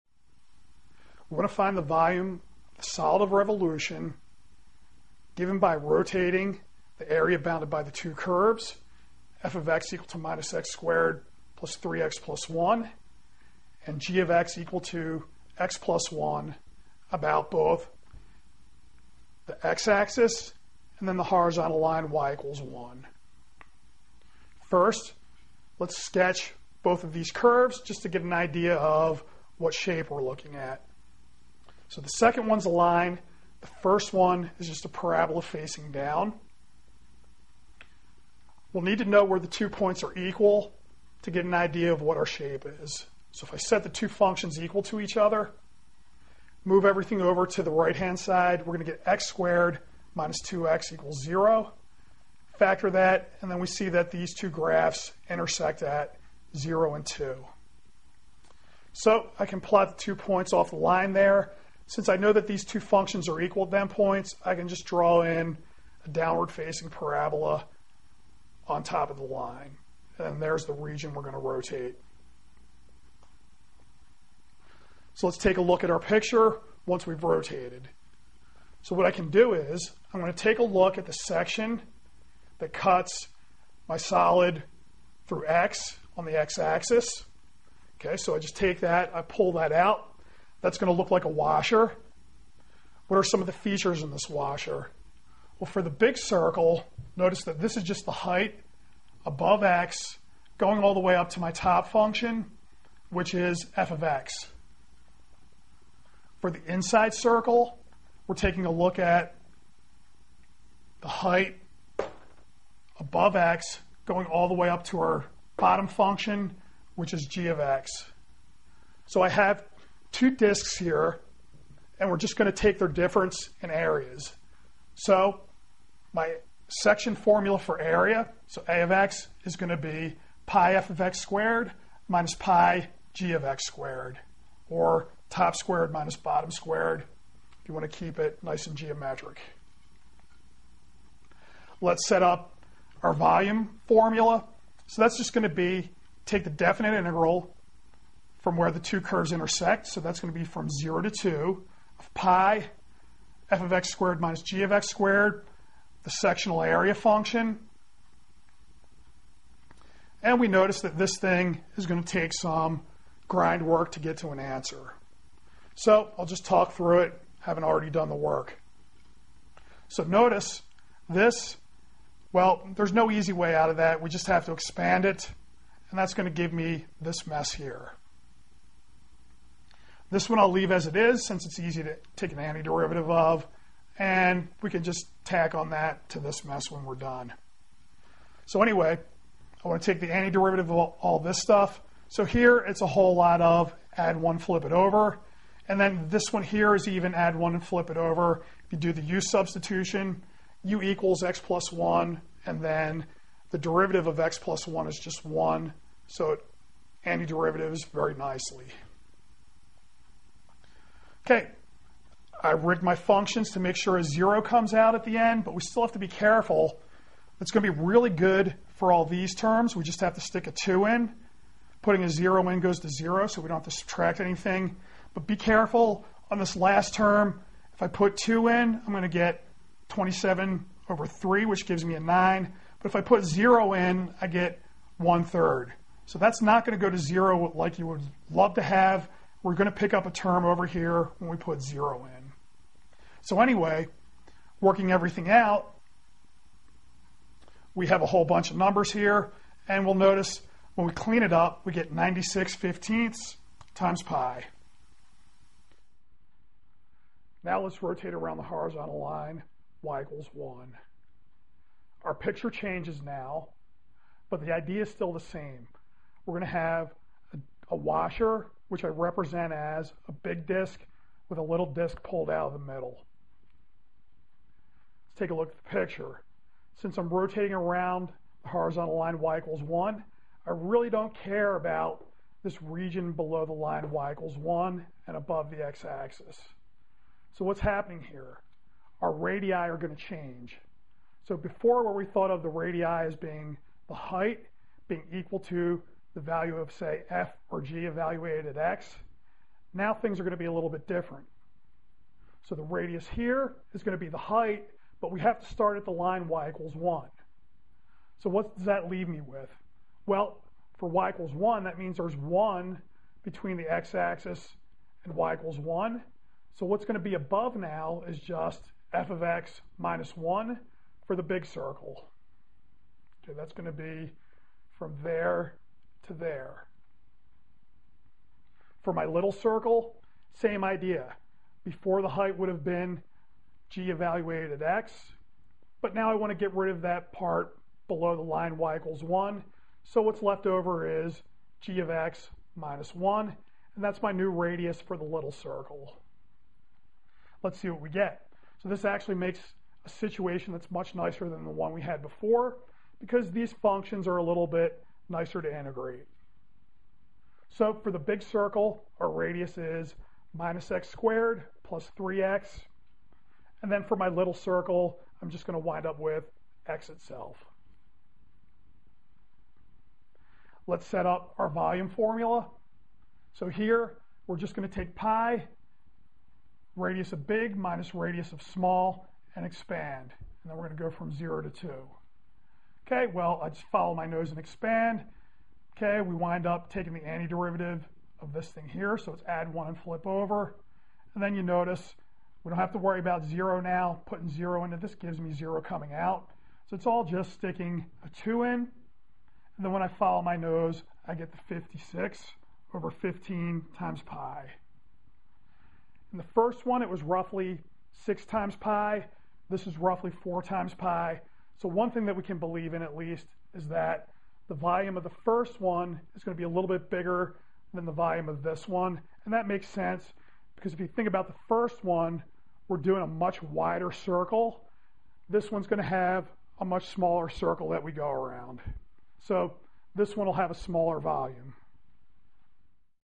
The Disk/Washer Method for Volume 1
Worked problem in calculus.   Let A be the area bounded by the curves  f(x) = -x^2 + 3x + 1  and g(x) = x+1.   The disk method is used to calculate the volume of the solid of revolution formed by revolving A about the (a) x-axis and (b) the line y=1.